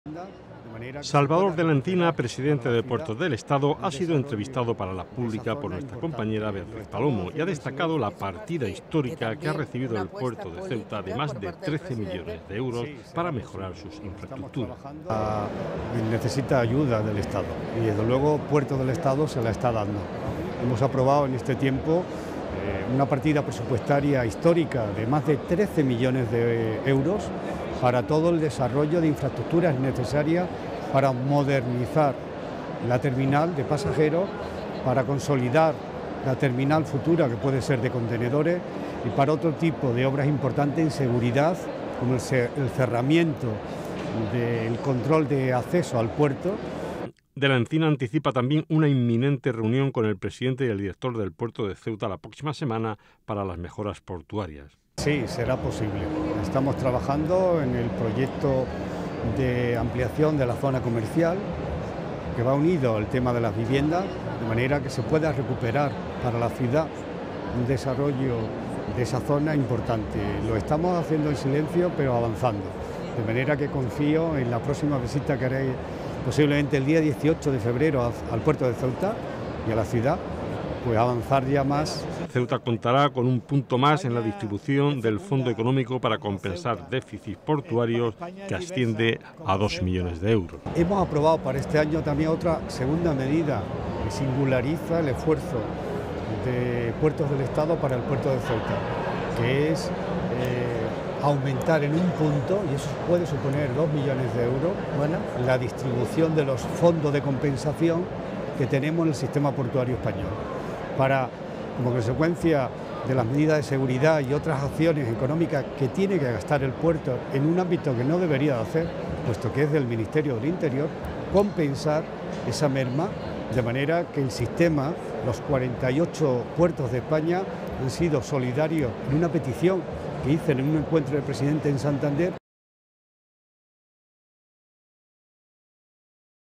De la Encina asegura que el puerto de Ceuta recibirá 2 millones
El presidente de Puertos del Estado ha anunciado a la Pública desde FITUR que nuestro puerto contará con un punto más en la distribución de fondos de compensación en el sistema portuario español, lo que supone 2 millones de euros más, como medida para singularizar el apoyo al puerto ceutí y que se unen a los más de 13 millones para el desarrollo de infraestructuras para modernizar la terminal de pasajeros, contenedores y seguridad.